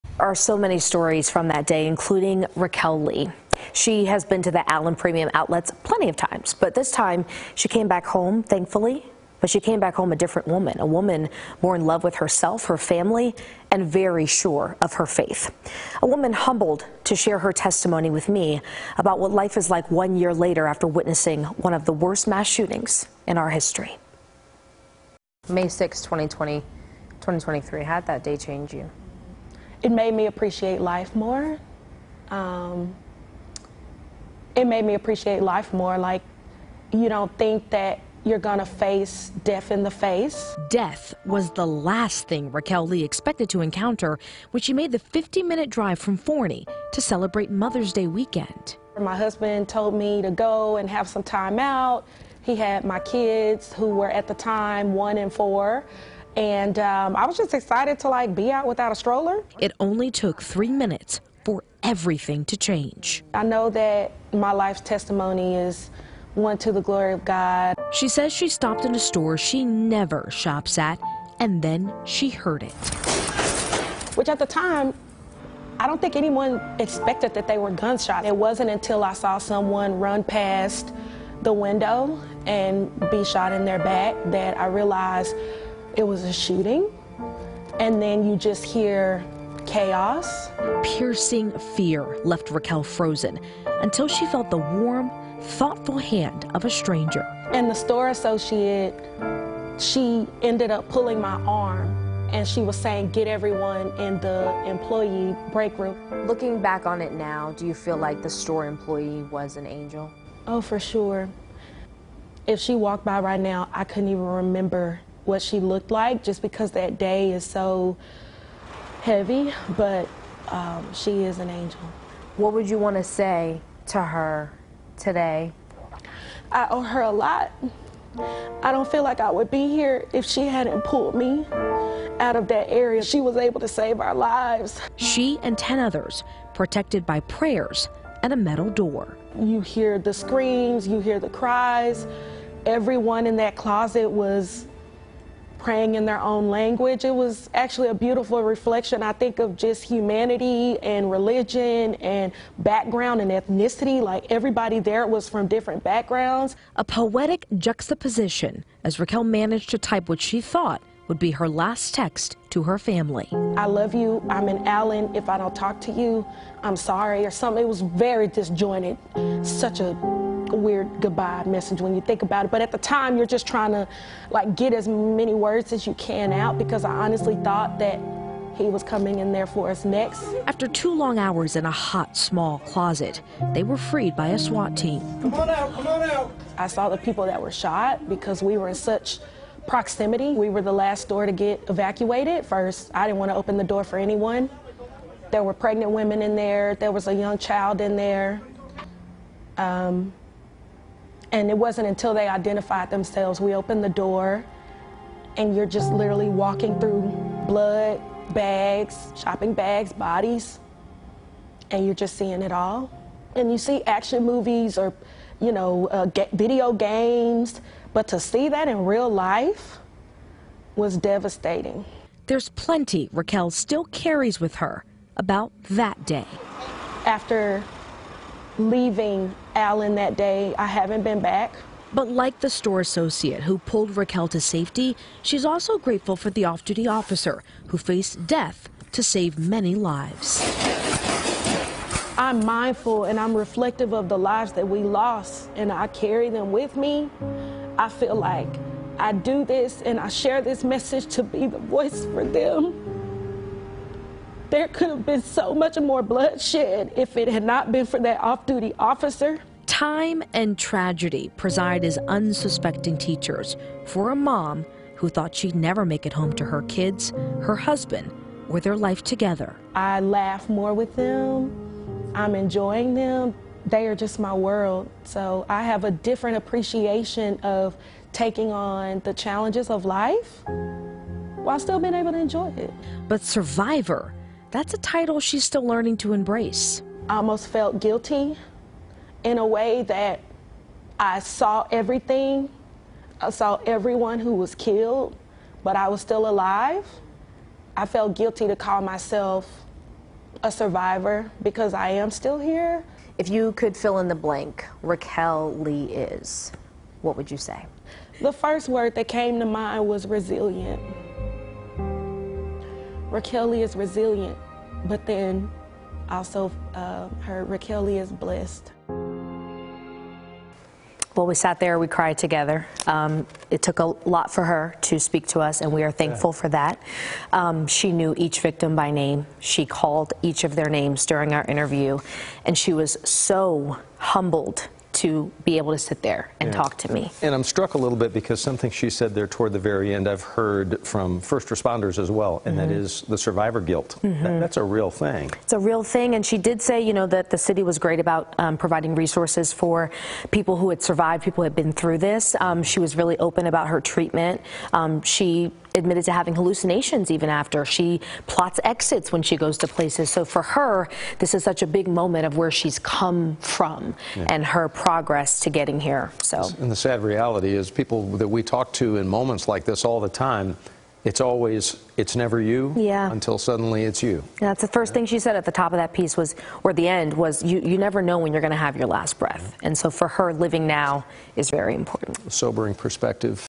Allen shooting survivor recalls gunfire, hiding and the woman who pulled her safety
"There was nowhere to run or go and I hadn't been in there long, and you start hearing gunshots."